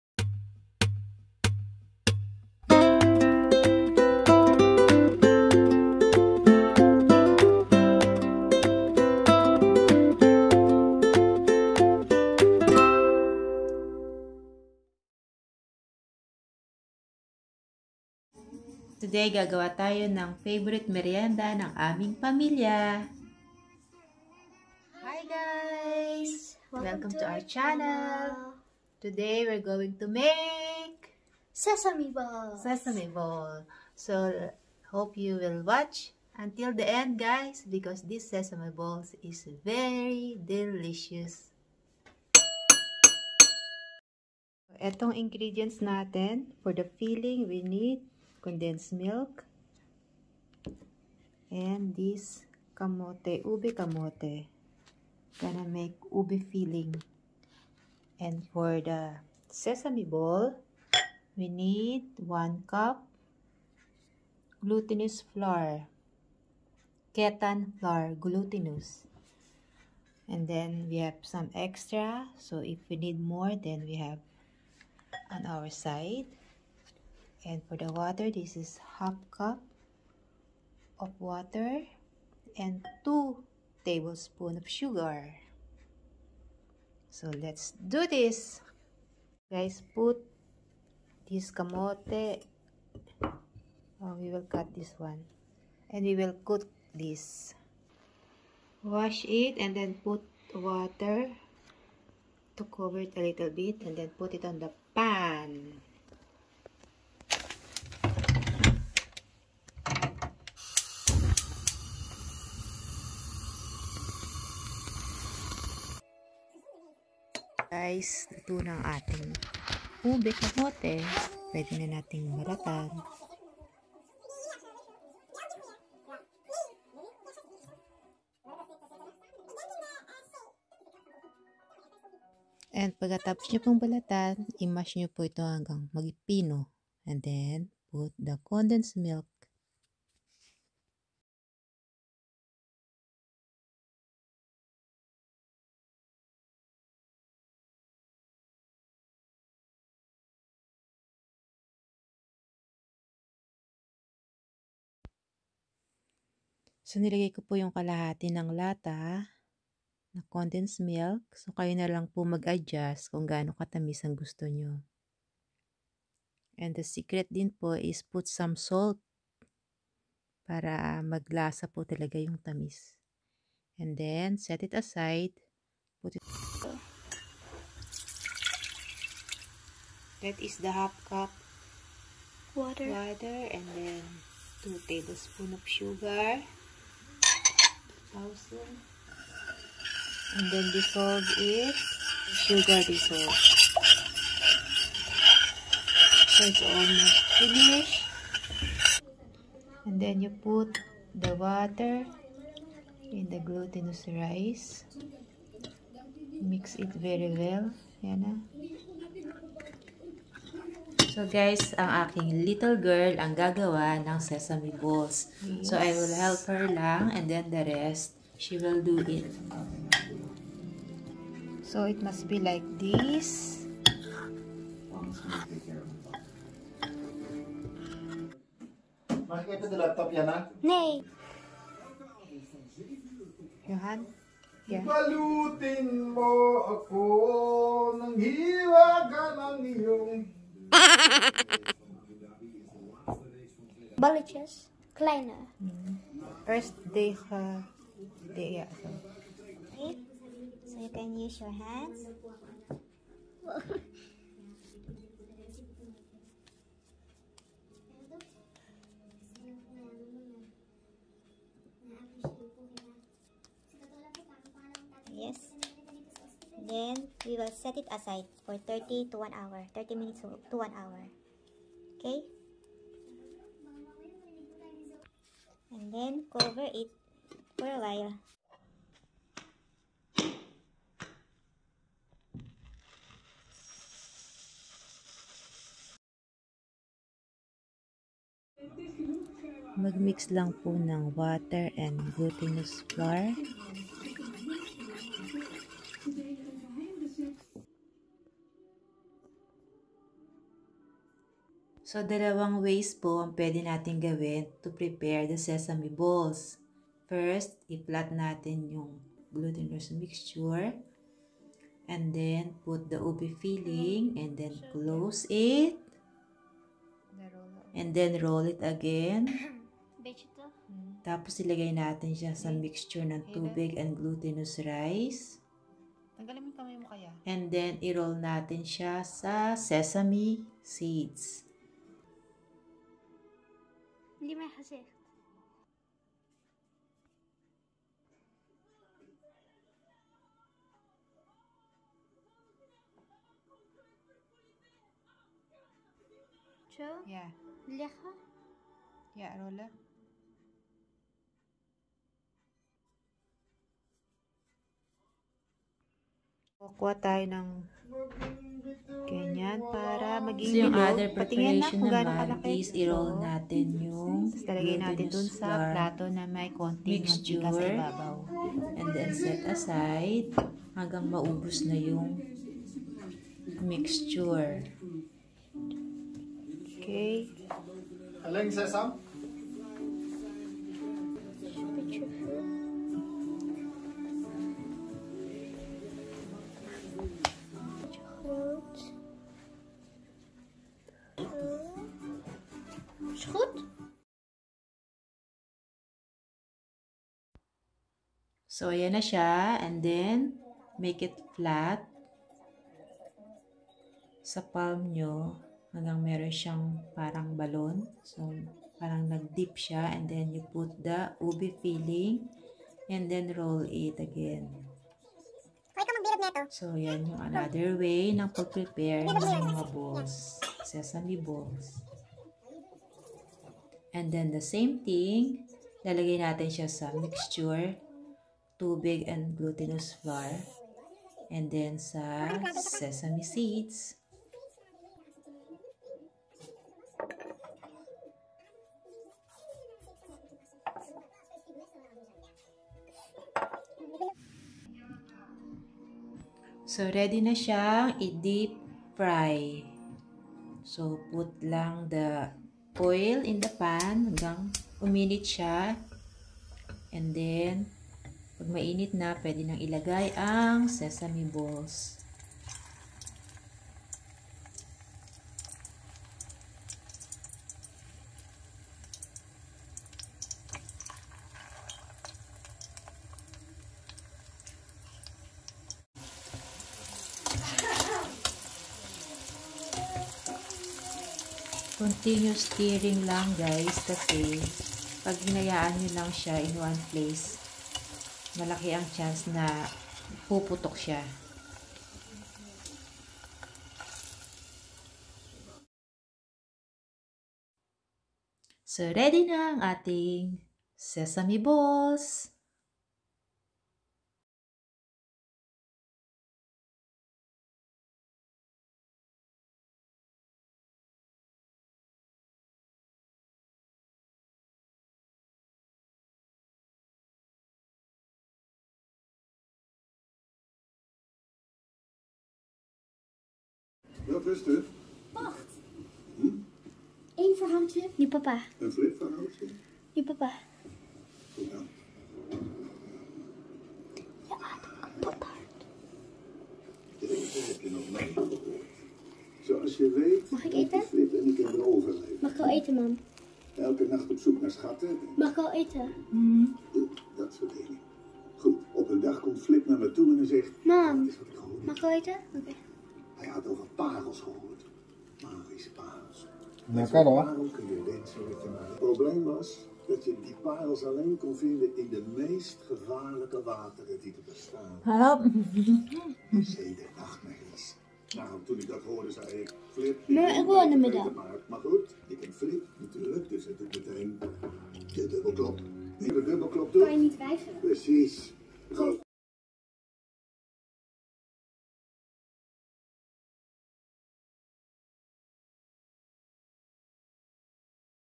SESAME BALLS | Favorite Snacks / dessert of the family | Easy to make and puede pang negosyo!!!!
In todays video we want to share with you the favorite snacks/ dessert of the our family. The Sesame balls, we used the ube filling because ube halaya is also one of our favorite.
This is what we used.
Glutinous Flour
Water
Sugar
Sesame seeds
Ube sweet potatoes
Condense milk
oil